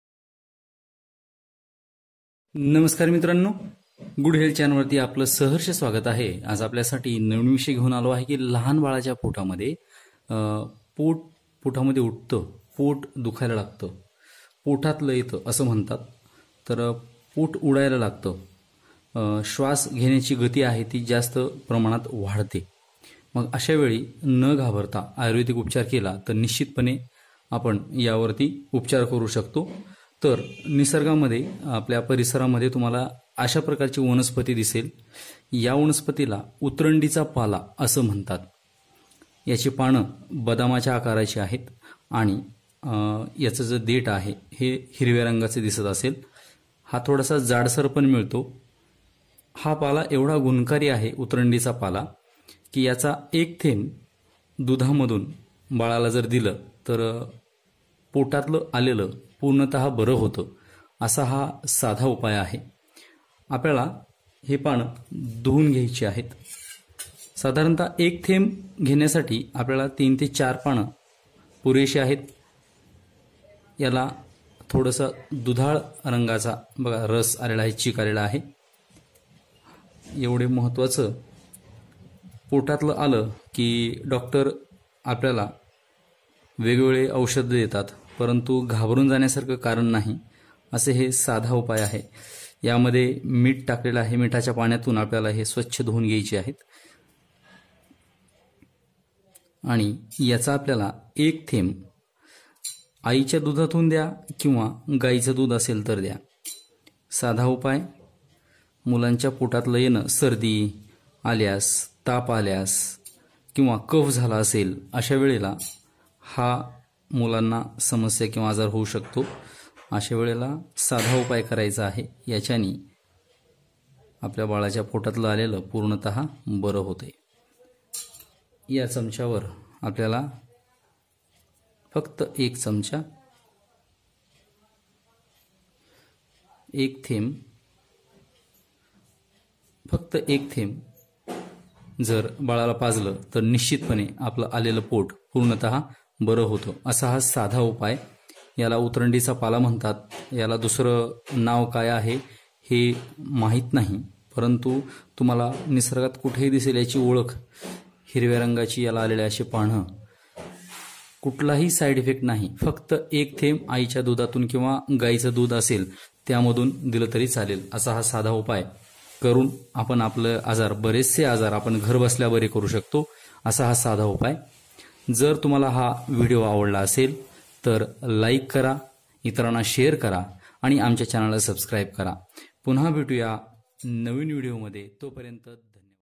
बाळाच्या पोटातलं आल्यास हा उपाय करा निश्चितपणे कमी होईल
सर्दी खोकला कफ झाल्यास बाळाचं पोट उडायला लागतं म्हणजेच पोटातलं येतं अशावेळी झाडपाल्याचा उपयोग केल्यास पोटातलं कमी होतं आयुर्वेदामध्ये झाडपाला महत्त्वाचं स्थान दिलं गेलं आहे आपल्या परिसरामध्ये उतरंडीचा पाला एवढा गुणकारी आहे की त्याचा एक थेंब जर बाळाला पाजला तर पोटातलं आलेलं पूर्ण बरं होतं असा हा आयुर्वेदिक गुणकारी उपाय